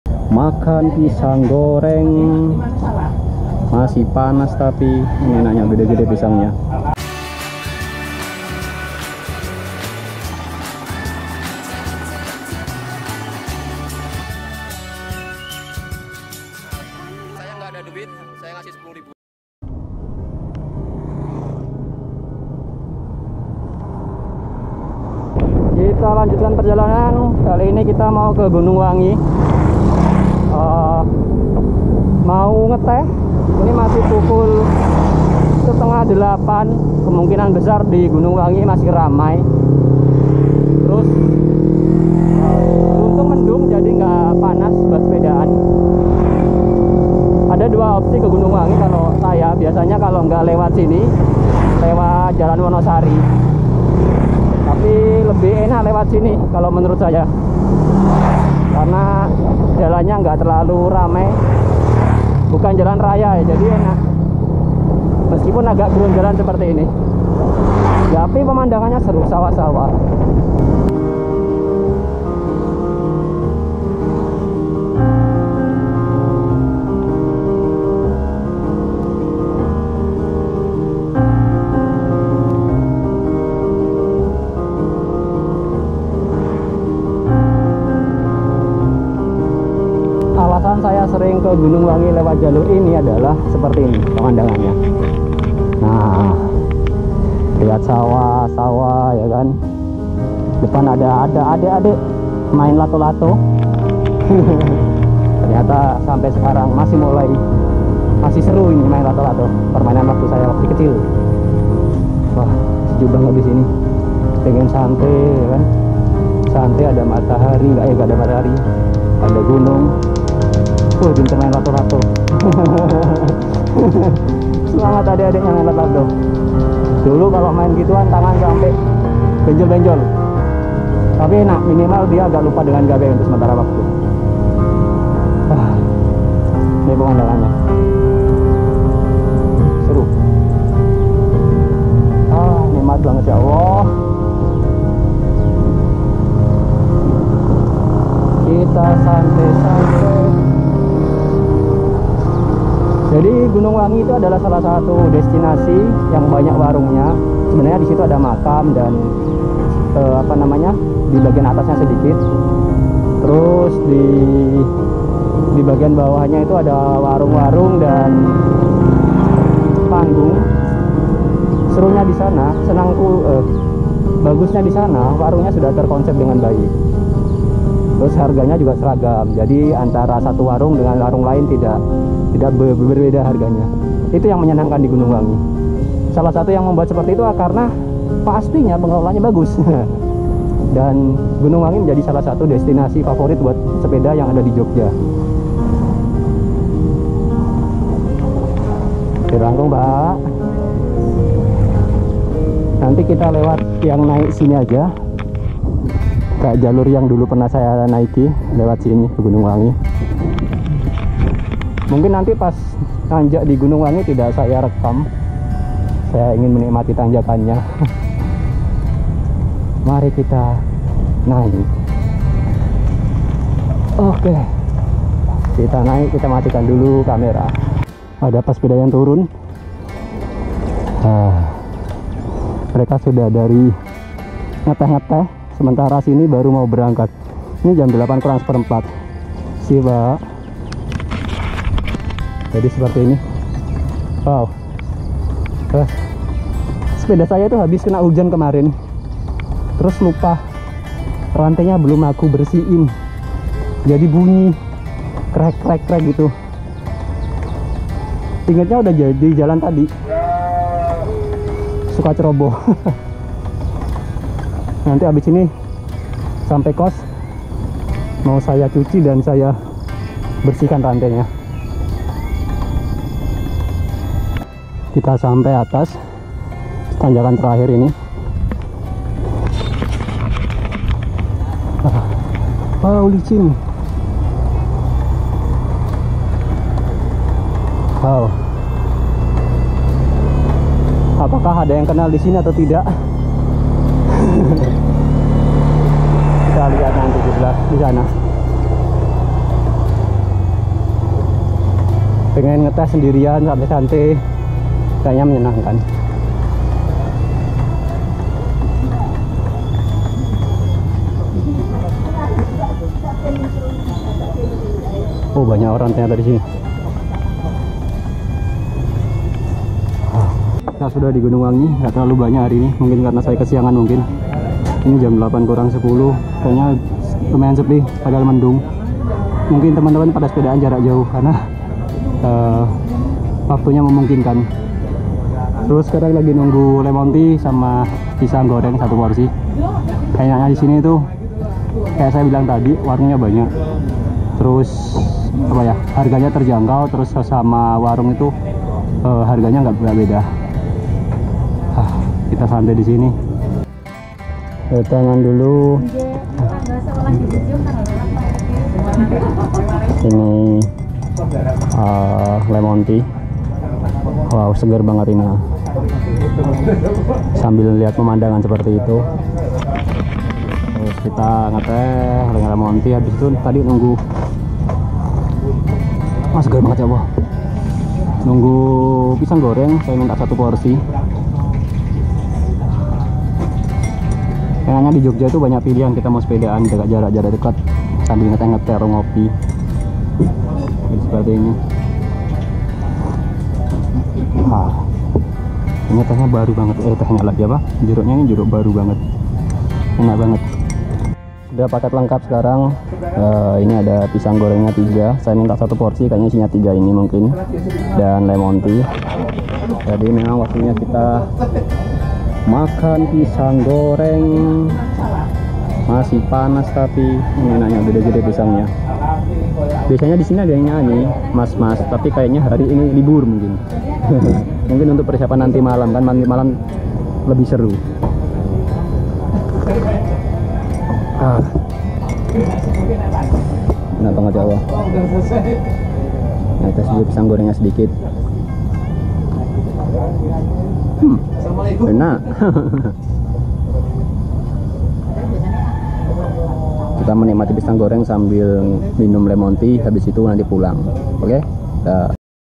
Menikmati Suasana Pagi di Bukit Gunung Wangi Bangkel
Kembali ke Gunung Wangi buat menikmati pisang goreng dan lemon tea. Ada yang suka lewat sini melalui jalan dari Candi Abang. Seperti ini pemandangannya